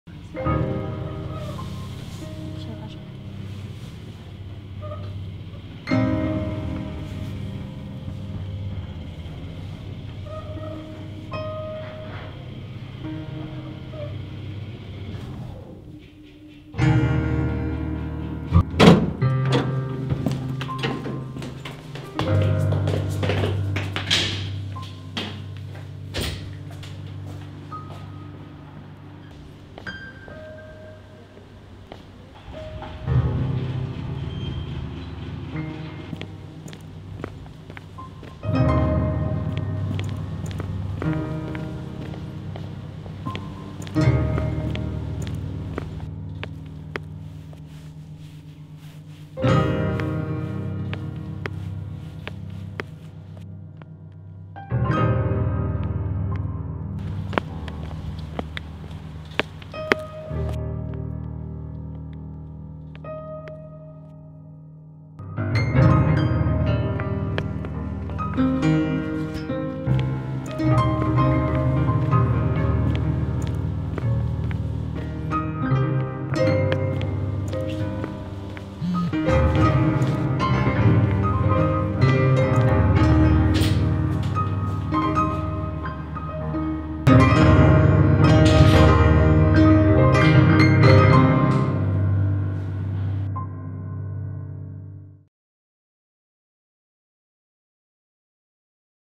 L A B I R Y N T
Kolejna odsłona filmów ćwiczeniowych zdjęciowo-montażowych 🙂
LABIRYNT - Joanna Szulc, Karol Hugo Pyczek, pod kierunkiem mgr Tymon Iwański